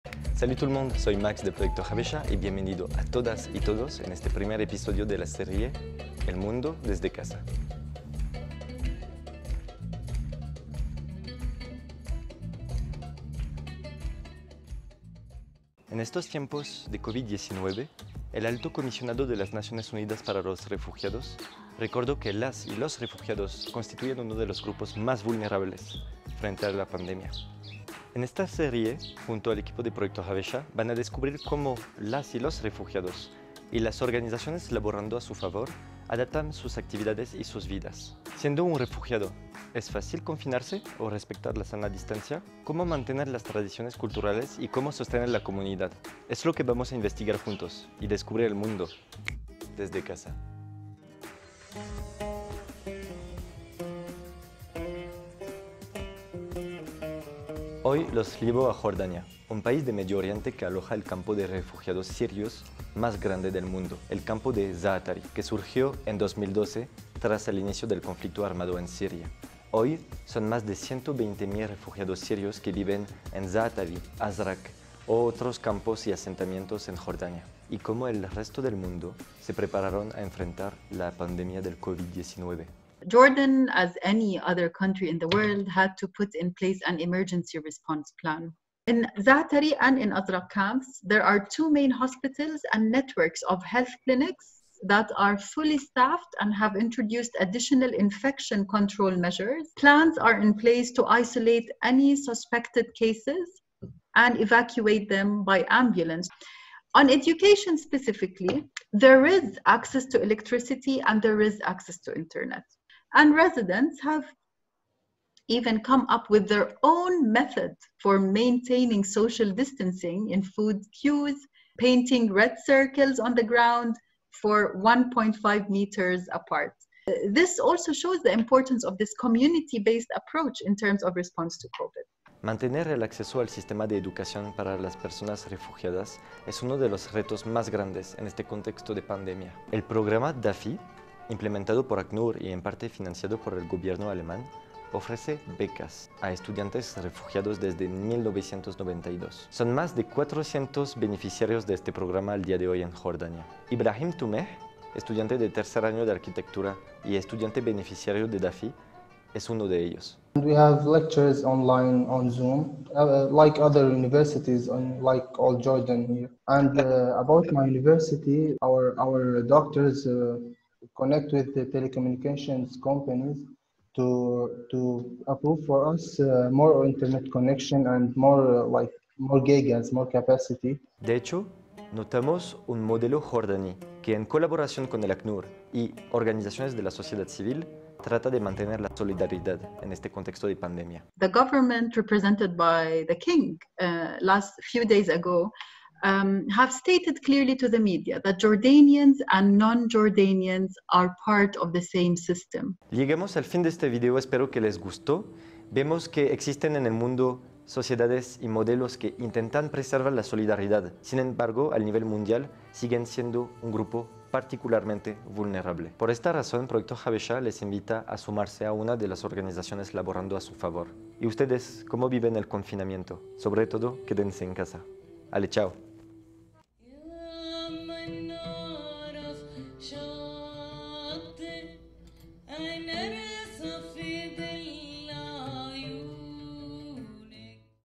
SER UN REFUGIADO EN JORDANIA EN TIEMPOS DE COVID-19 | "El Mundo Desde Casa" | Episodio 1
¿Es fácil confinarse viviendo en un campo de refugiados? ¿Cómo mantener en este caso su acceso a los servicios básicos? ¿Es posible acceder a la educación digitalizada? En este primer episodio de la serie "El Mundo Desde Casa", les llevamos a Jordania, para descubrir cómo las y los refugiados, al igual que las organizaciones internacionales y la sociedad civil lograron mantenerse solidarios en tiempos de COVID-19.